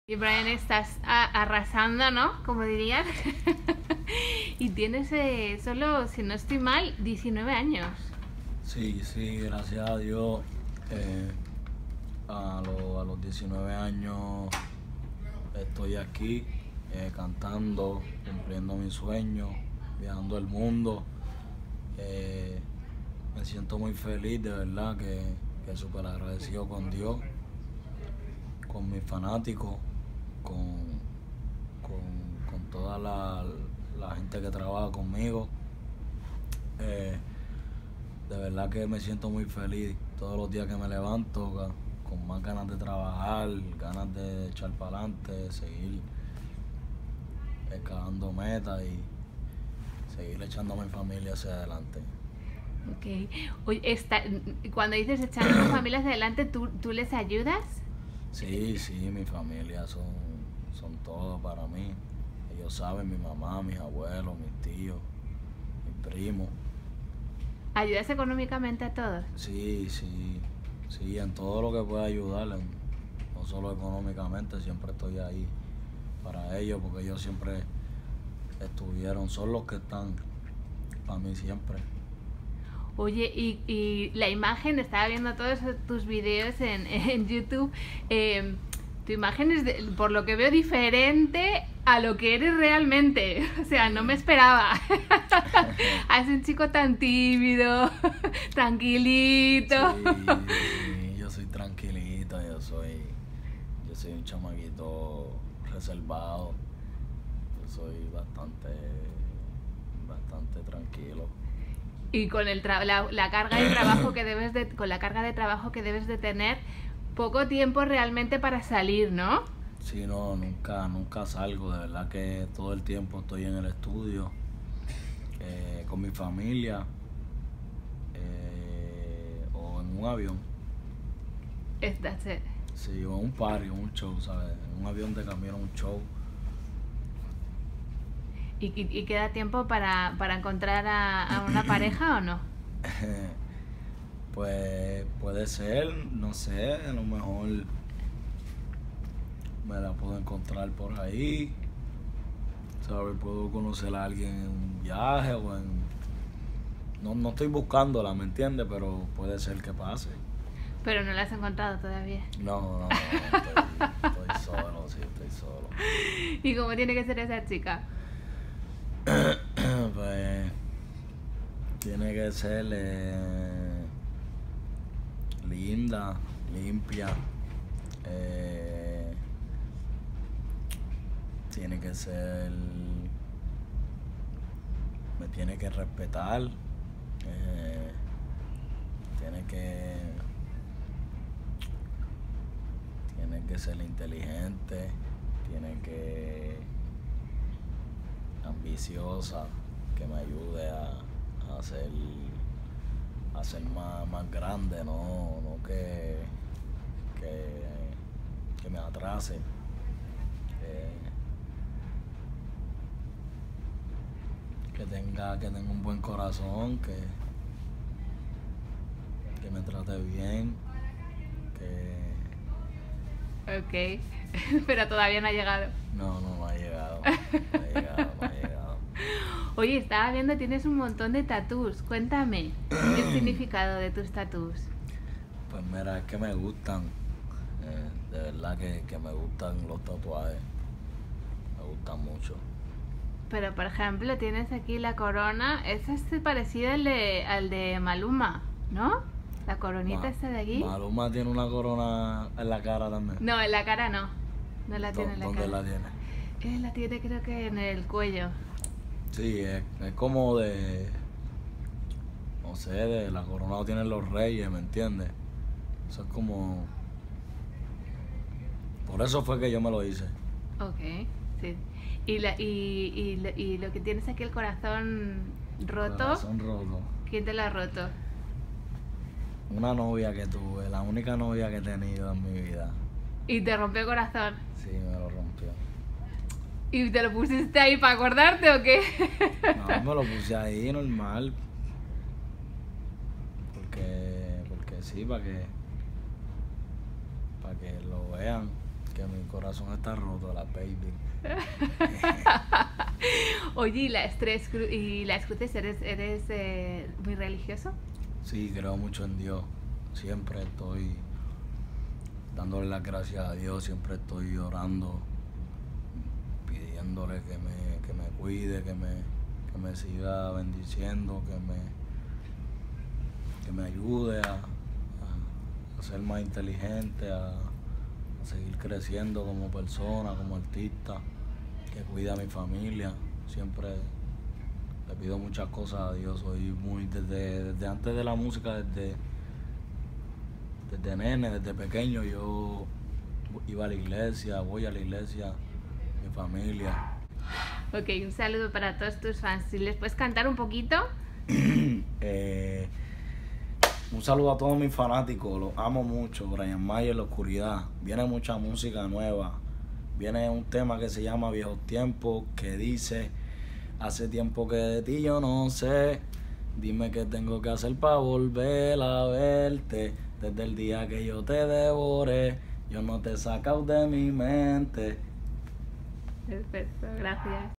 Bryant Myers tiene el corazón roto por su única novia. SIN EDITAR
No dejes de sorprenderte con esta entrevista, sin editar, al cantante puertorriqueño Bryant Myers. Conoce su personalidad detrás de cámaras y no te olvides decirnos qué te pareció.